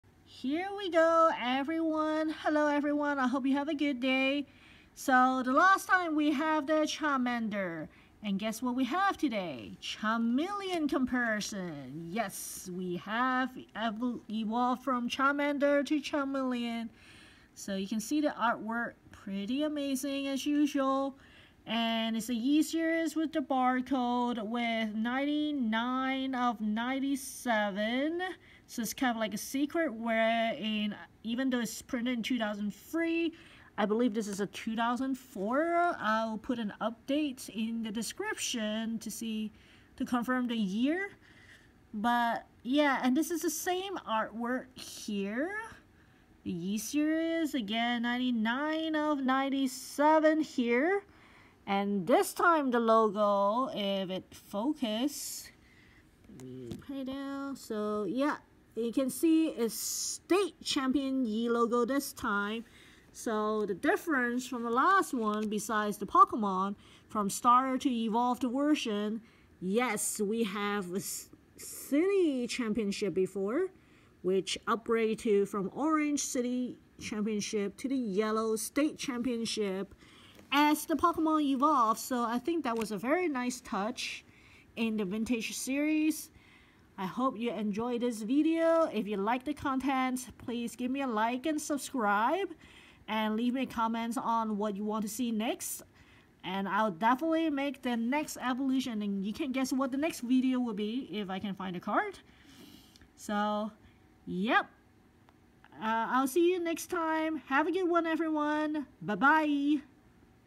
Vintage Pokemon card Charmeleon State Championships 2004
Charmeleon リザード  (99/97) was awarded to participants of the State Championships 2004 in North America during Apr 3, 2004 - May 2, 2004.

This card is the same design as that in EX Dragon 天空の覇者 Rulers of the Heavens, and the card is illustrated by Mitsuhiro Arita.  The stamped logo makes it rare.